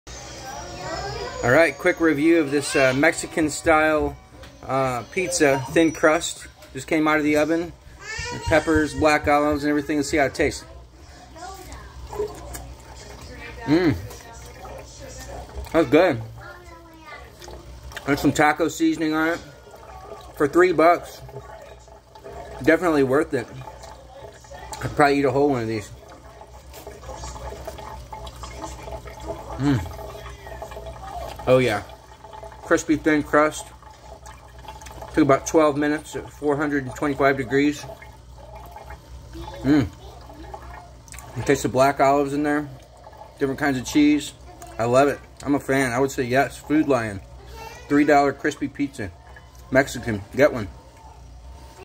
Food Lion Frozen Mexican Pizza Review
Great quick lunch at home. Leftovers go in the Hot Logic for lunch in the truck.